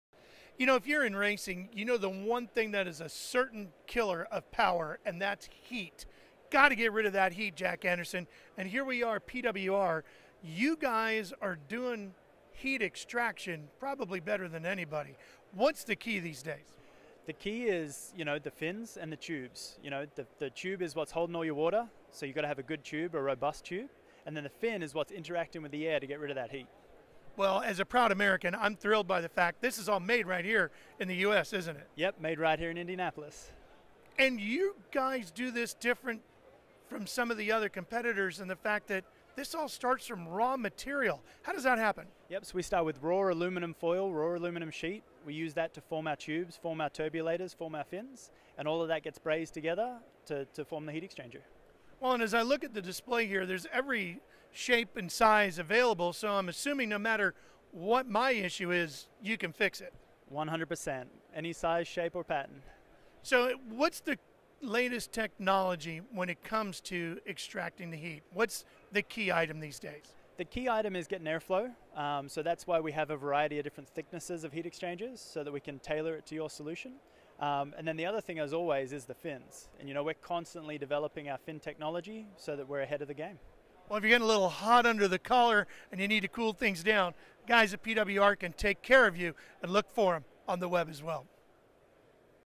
PWR Radiators and Intercoolers
Hear Jack Anderson talk about PWR and our top of the line cooling products.
PWR offers a large variety of heat exchangers. From radiators and intercoolers, to supercharger bricks and complete custom cooling packages, we have you covered!